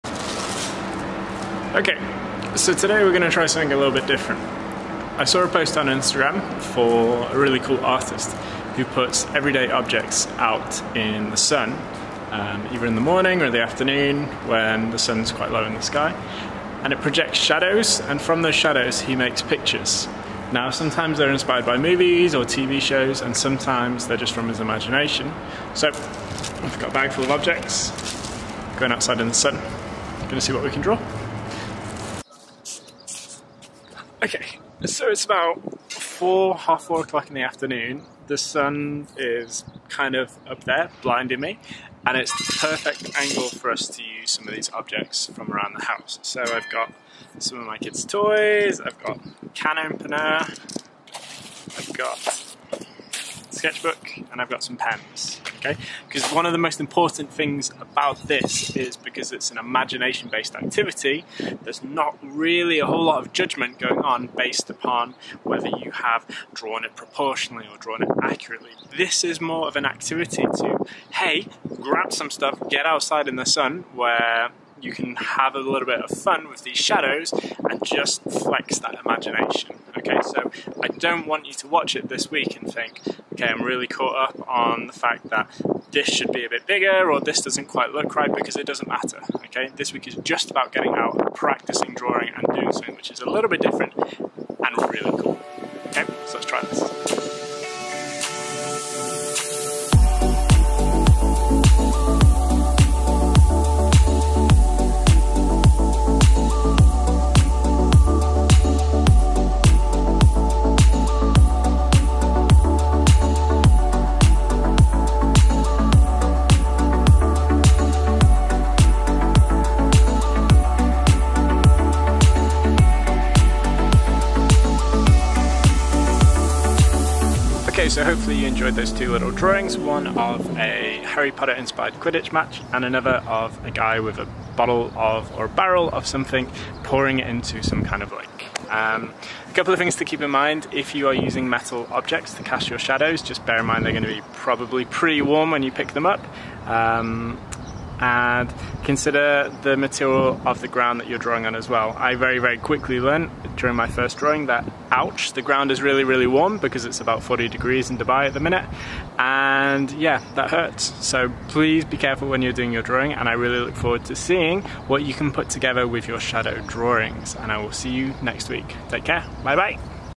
Drawing with Shadows!
This week, we look at the work of Vincent Bal, who creates amazing drawings combining objects in the morning or afternoon sun!

Grab some household objects, some paper and a pen and come and join me this week as I brave the 40 degree Dubai sun to create some imagination based drawings!